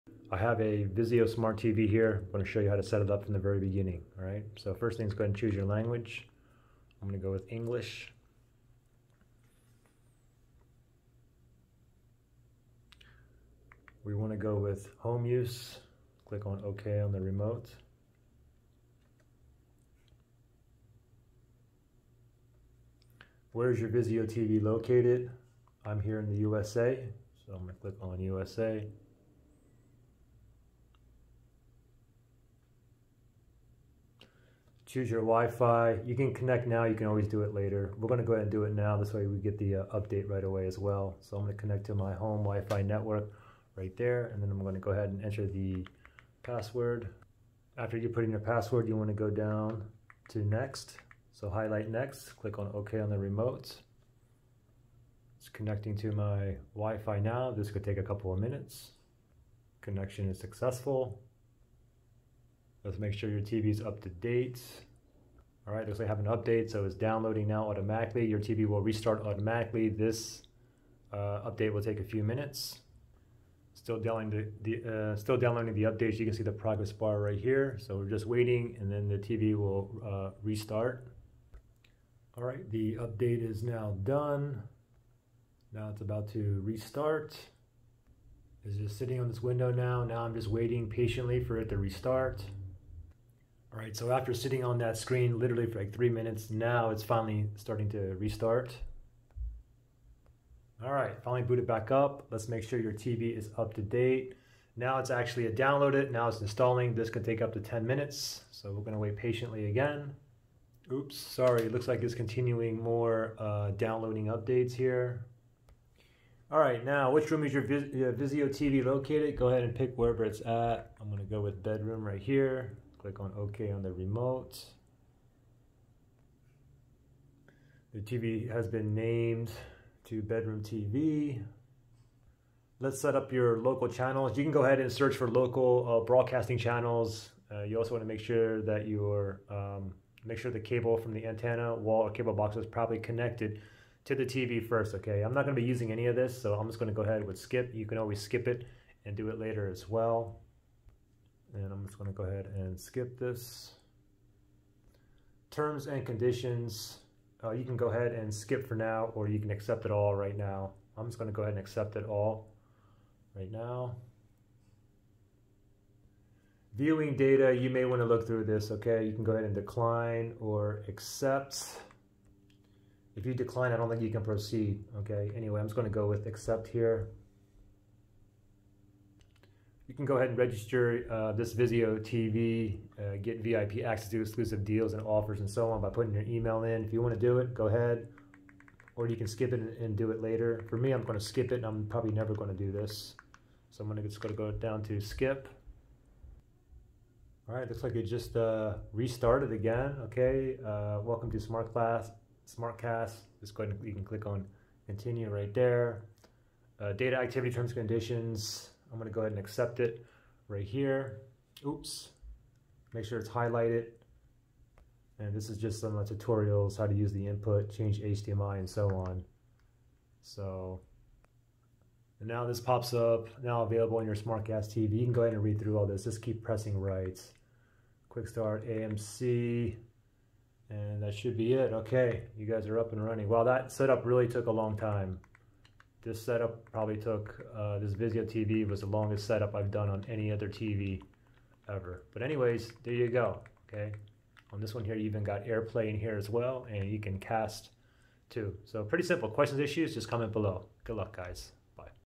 Vizio Smart TV: How to Setup for Beginners (step by step)
I show you how to setup the Vizio smart tv step by step right out of the box. I hope this helps.